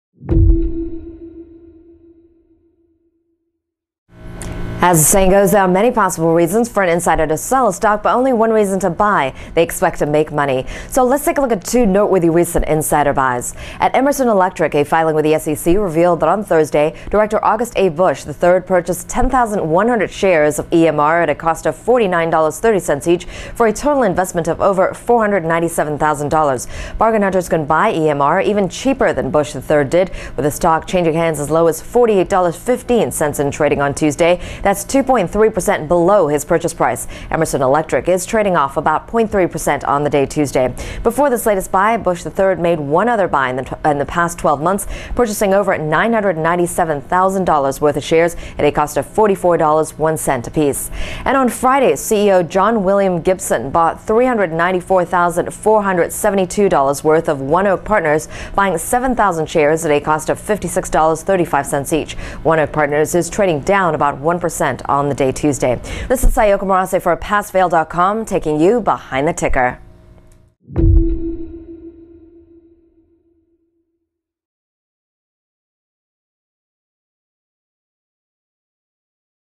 Passfail.com News: Tuesday 5/8 Insider Buying Report: EMR, OKS
As the saying goes, there are many possible reasons for an insider to sell a stock, but only one reason to buy -- they expect to make money.  So let's look at two noteworthy recent insider buys.

At Emerson Electric Co. (EMR), a filing with the SEC revealed that on Thursday, Director August A. Busch III purchased 10,100 shares of EMR, at a cost of $49.30 each, for a total investment of $497,929.  Bargain hunters can buy EMR even cheaper than Busch III did, with the stock changing hands as low as $48.15 in trading on Tuesday -- that's 2.3% below Busch III's purchase price.  Emerson Electric Co. is trading off about 0.3% on the day Tuesday.  Before this latest buy, Busch III made one other buy in the past twelve months, purchasing $997,233 shares at a cost of $44.01 a piece.

And on Friday, CEO John William Gibson bought $394,472 worth of ONEOK Partners LP (OKS), buying 7,000 shares at a cost of $56.35 each.  ONEOK Partners LP is trading down about 1% on the day Tuesday.